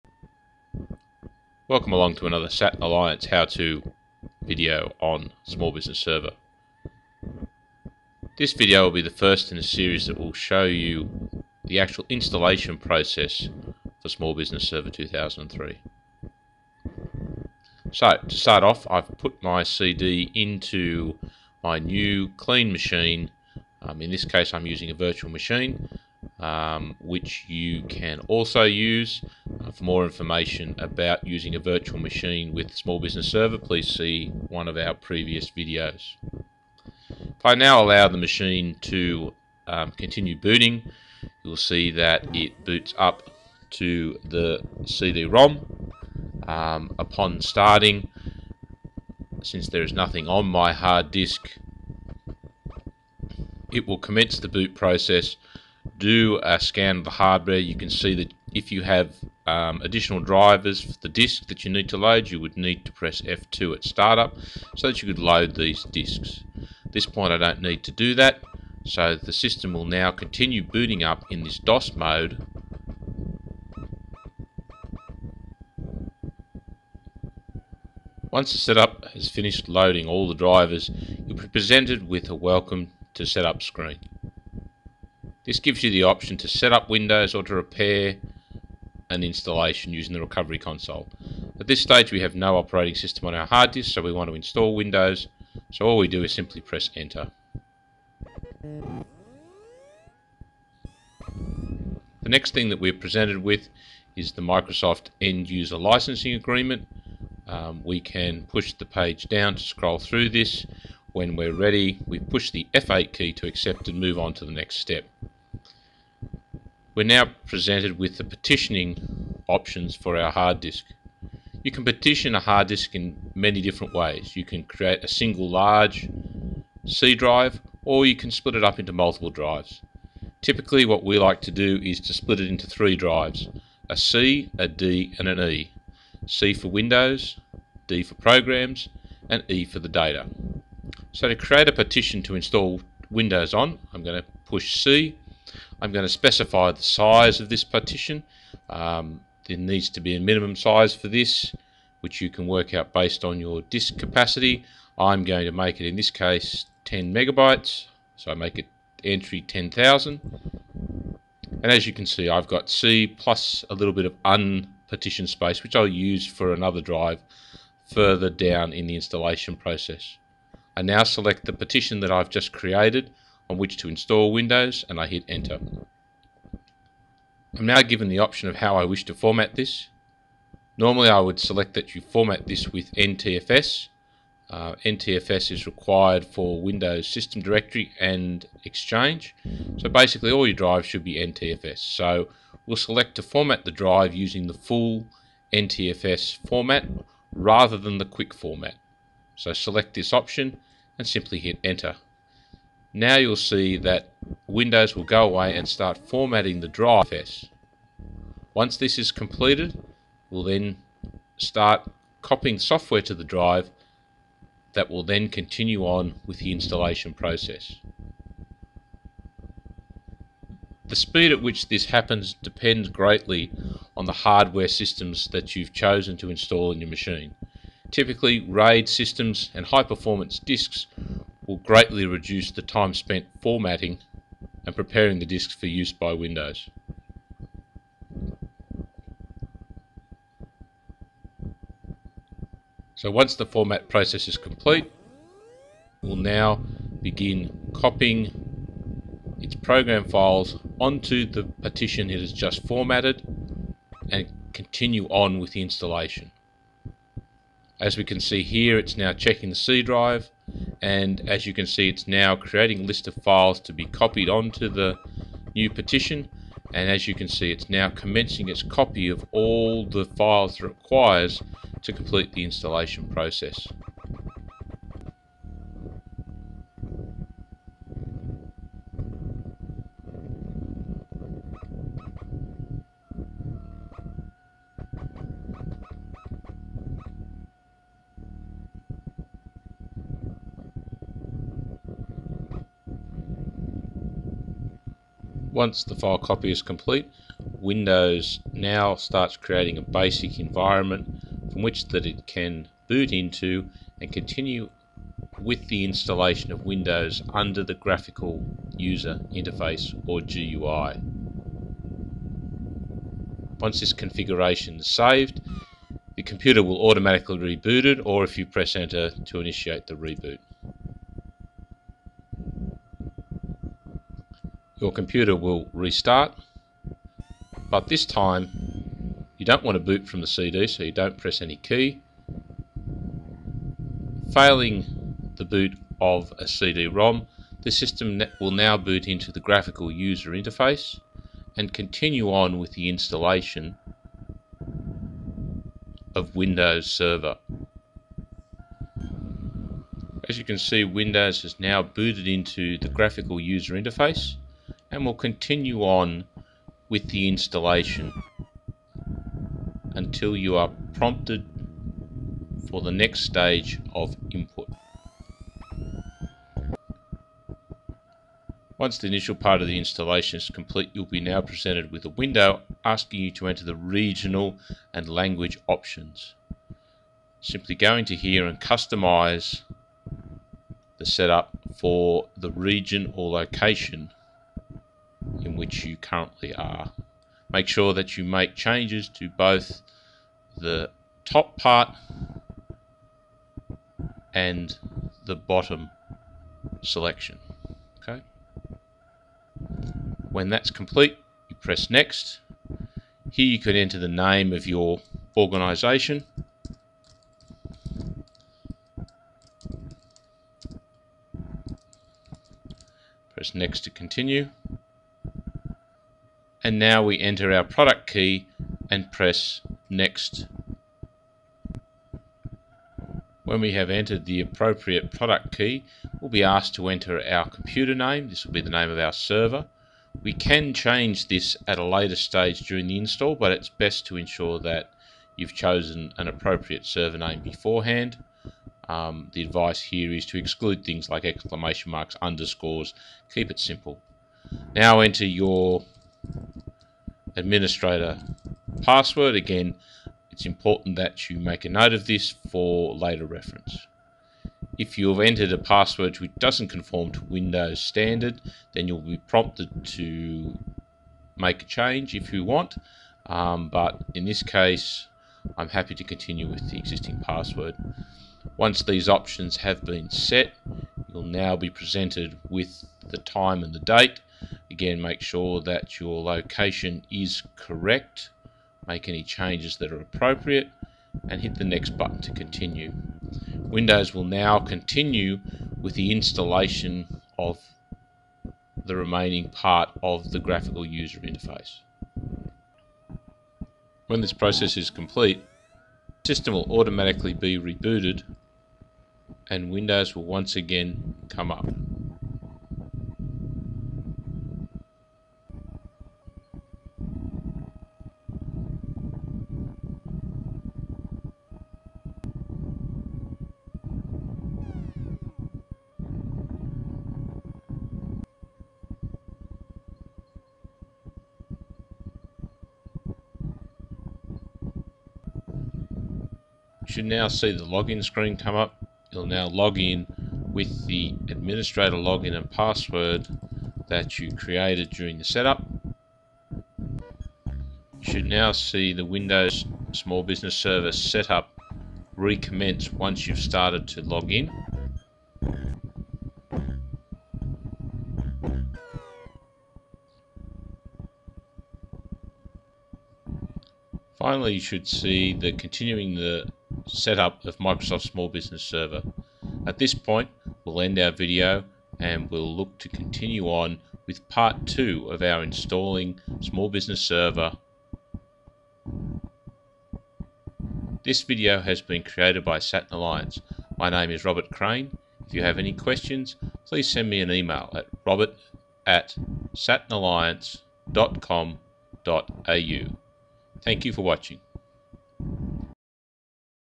SBS 2003 Installation Part 1
This series of videos will run through the installation of Small Business Server onto new hardware. This first video covers the initial setup in the Blube Screen or DOS environment all the way through getting the Windows Graphical User Interface (GUI) installed.